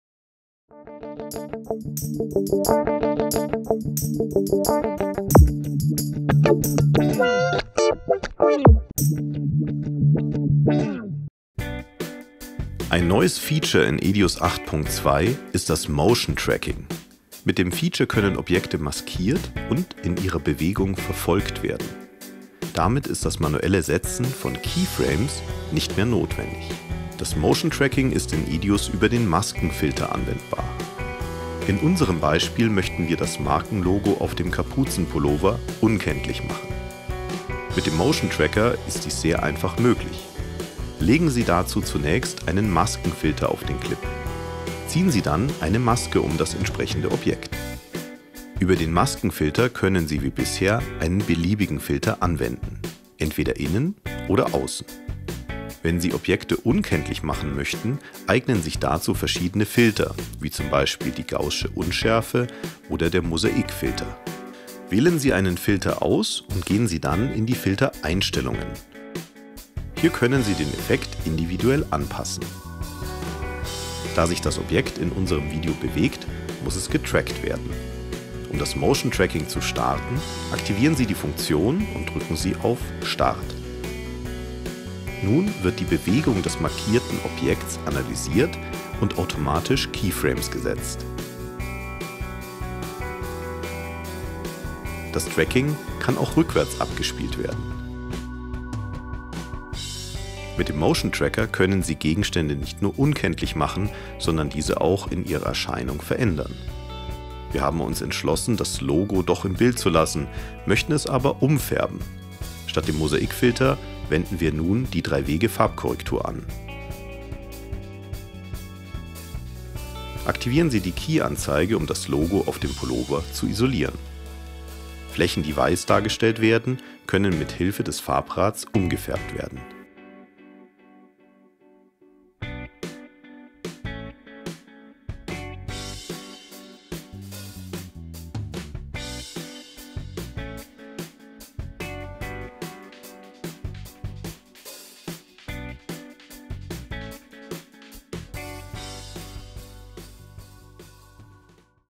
EDIUS Podcast - EDIUS 8.2 Motion Tracker
In diesem Video stellen wir Ihnen eine Neuerung in EDIUS 8.2 vor - den Motion Tracker. Mit dem Tool können Sie Objekte im Video maskieren und in ihrer Bewegung verfolgen.

Weitere Informationen, Tipps und Tricks, die kostenlose 30 Tage Testversion und mehr erhalten Sie